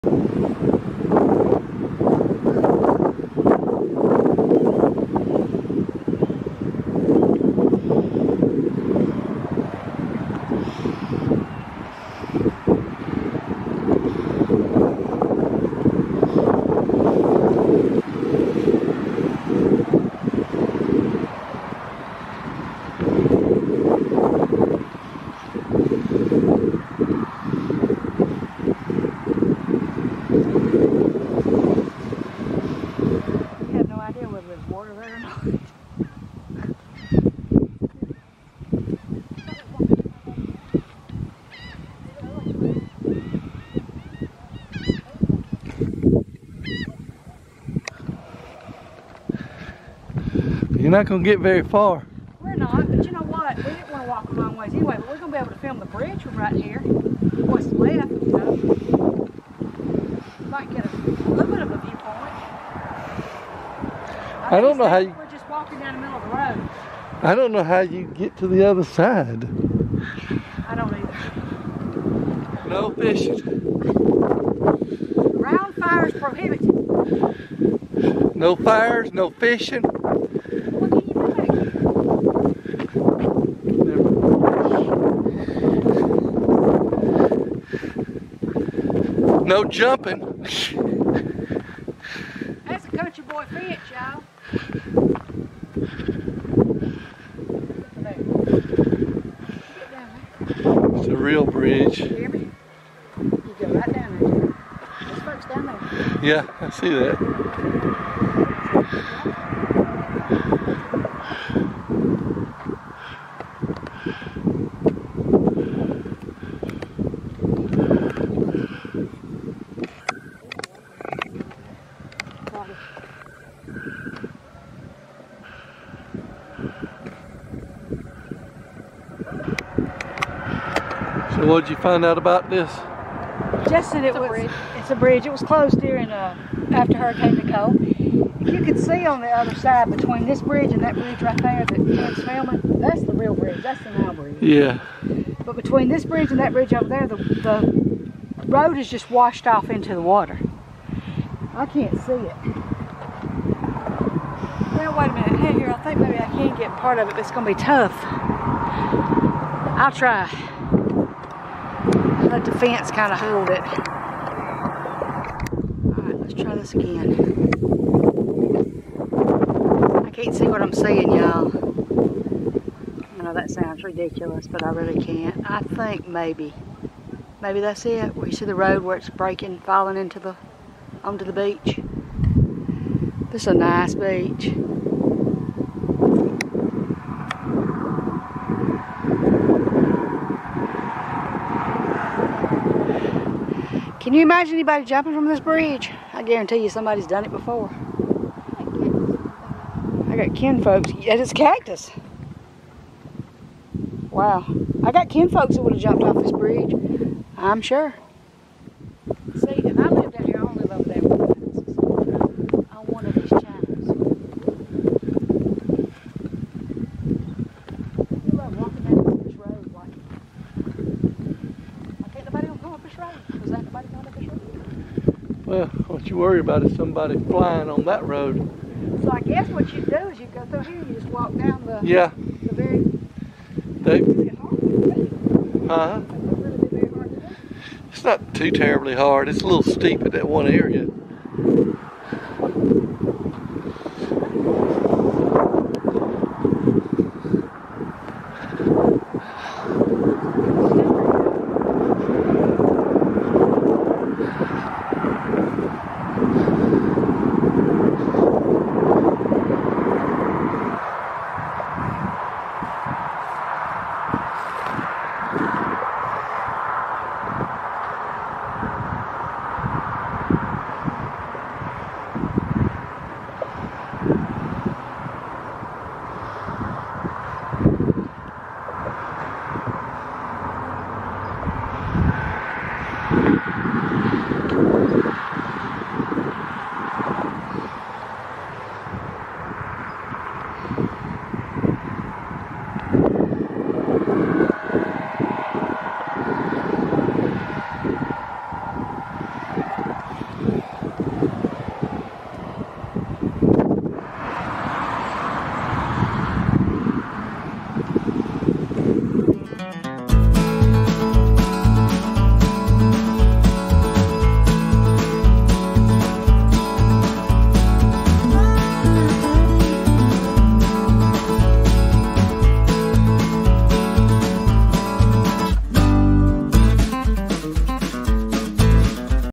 Big Talbot Island State Park - Crady Bridge Florida! Parts of the Road Washed Away During Hurricane!
We crossed the Florida line for a little while on our last big trip and found Crady Bridge Fishing Pier! As we rode across the new bridge and looked to the side at the old bridges, we saw where the road had been partially washed away. With research we found out it happened when Hurricane Nicole came through, but then when we got home and looked at this further we realized there have been several Hurricane Nicoles over the years.

If you would like to help fuel our adventures (and I literally mean help buy gas :-) please consider becoming a Patron here: